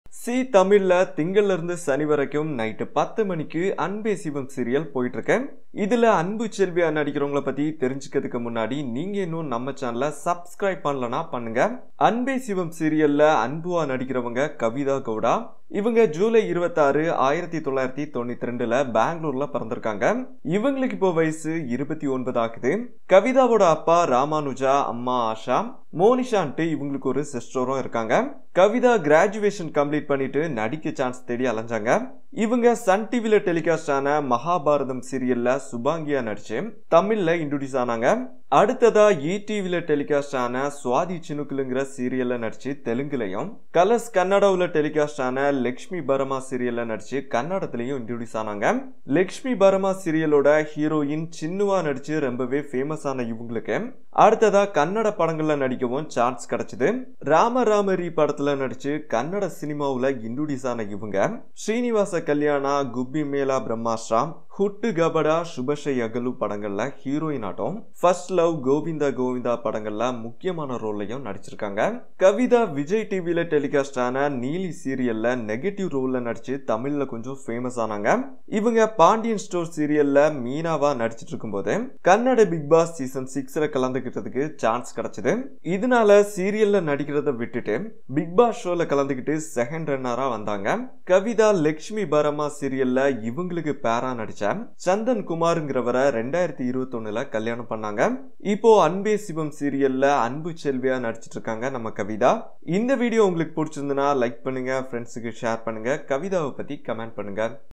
அன்பே சிவம் சீரியல் ஹீரோயின் கணவர்? | Anbe Sivam serial heroine Anbu Selvi (Kavitha Gowda) Biography
அன்பே சிவம் சீரியல் ஹீரோயின் நிஜ கணவர்? | Anbe Sivam serial heroine Anbu Selvi Biography wiki, birthday date, age, parents, marriage, husband name, biodata, movies, serials, lifestyle.

// Video Content //
00:11 Anbe Sivam serial Anbu Selvi real name
00:15 Kavitha Gowda date of birth
00:20 Kavitha Gowda age
00:22 Kavitha Gowda family
00:32 Kavitha Gowda serials
00:57 Kavitha Gowda movies
01:30 Kavitha Gowda husband name

These pictures are not mine , and it taken from net